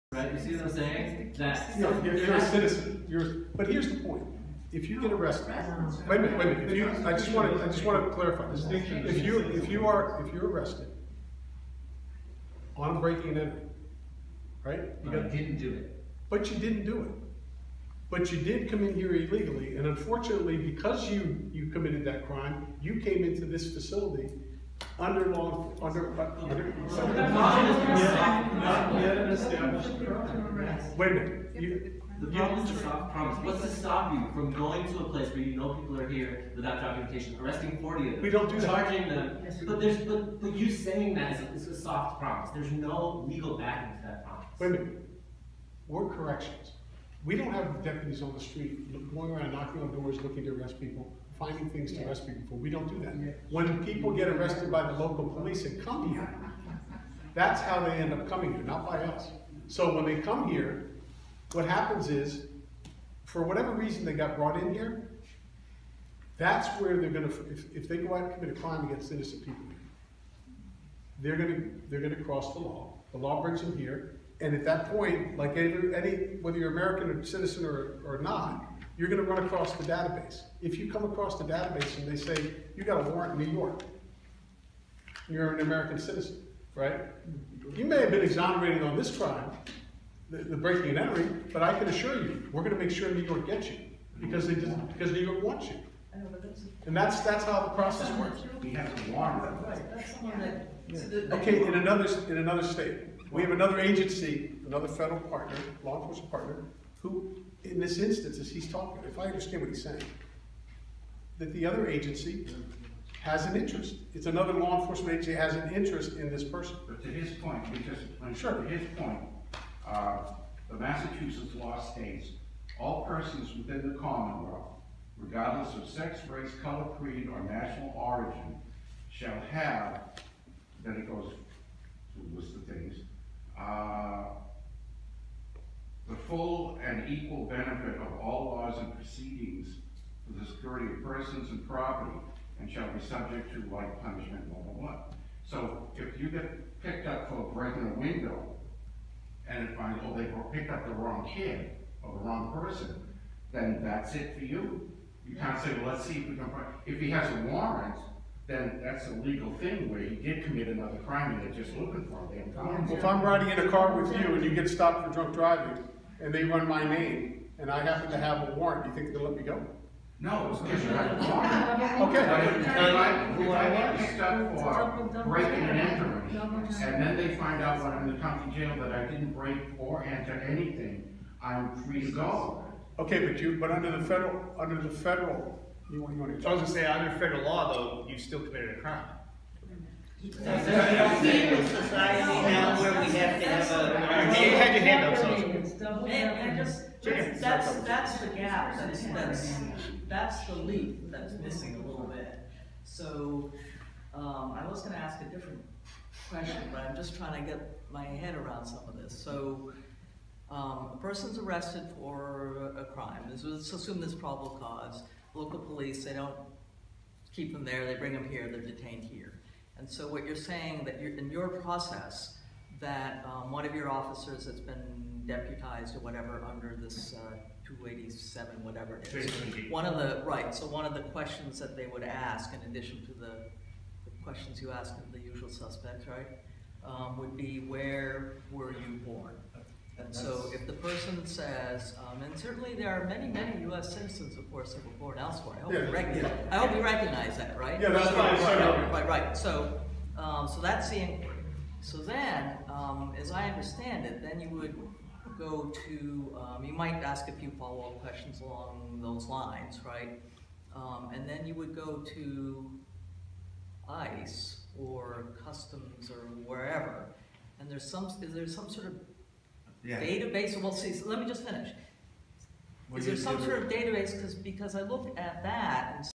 Sheriff Hodgson on his partnership with ICE
Bristol County Sheriff Thomas Hodgson speaks at a Jan. 11 public meeting about his contract to deputize his officers to do ICE immigration work.